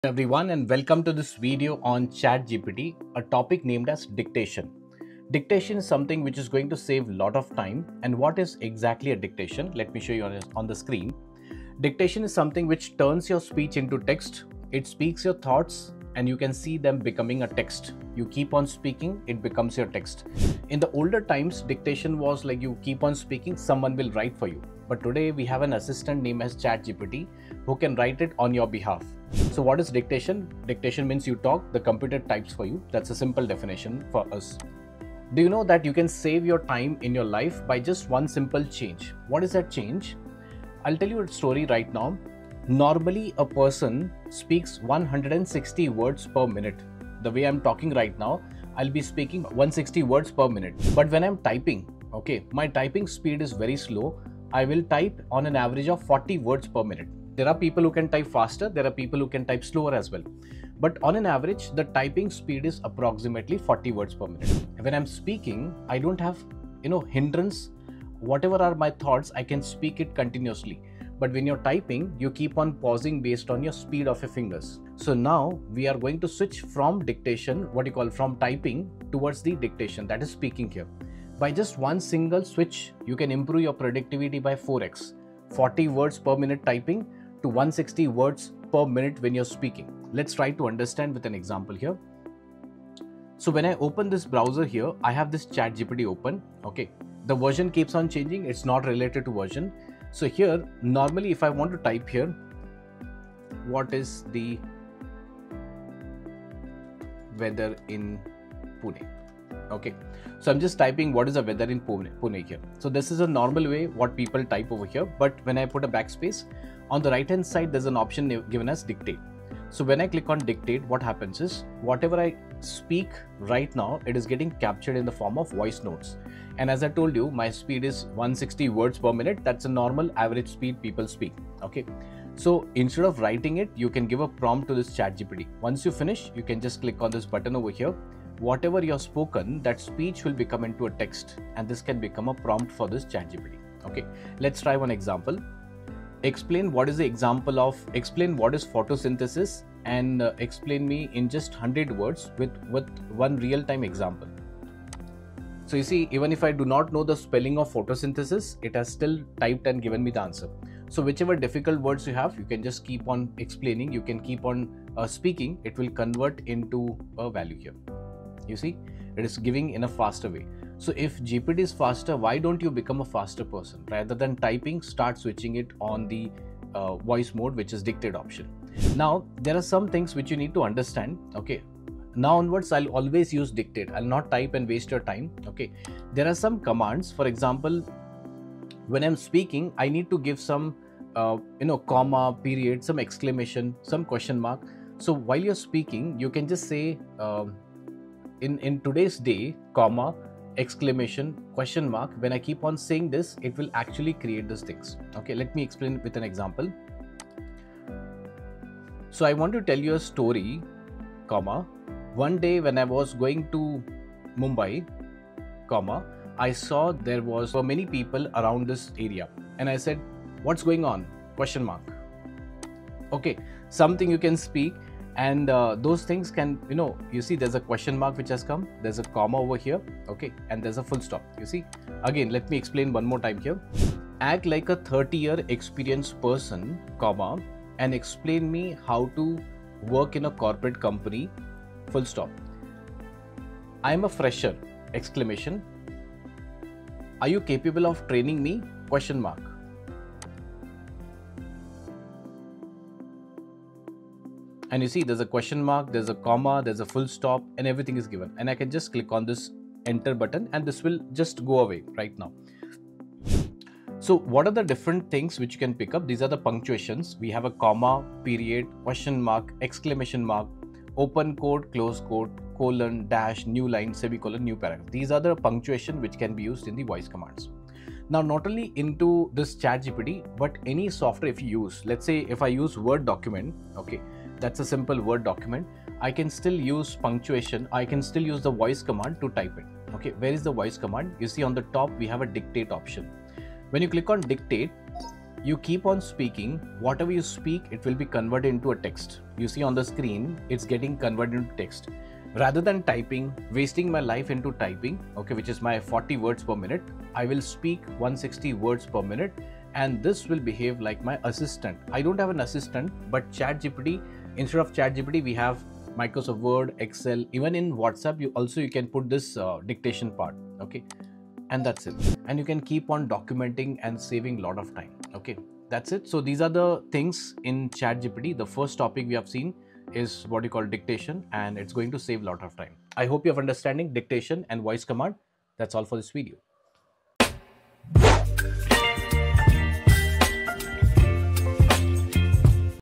The Hidden Switch that Makes ChatGPT 4X Faster
⌨️ Tired of typing long prompts into ChatGPT?

Your keyboard is slowing you down!
 Stop typing slowly and start talking to ChatGPT 4X FASTER using this simple,
 hidden dictation secret.

You speak at 160 WPM, but type at only 40 WPM.
 Learn the exact feature that unlocks your full speed, letting you create complex, detailed AI prompts in seconds saving hours of work!

In this quick guide, you'll master:
1. How to instantly enable voice dictation in ChatGPT.
2. Essential voice commands for perfect punctuation.
3.How this hack works in apps like MS Word.

Watch now to instantly upgrade your workflow and start working at the speed of thought!

💬 Dictation isn't new but using it with ChatGPT is a productivity game-changer.

🎯 Perfect for:
✅ Students
✅ Creators
✅ Busy professionals
✅ Anyone who wants to work faster

✅ 100% practical
✅ Beginner to Pro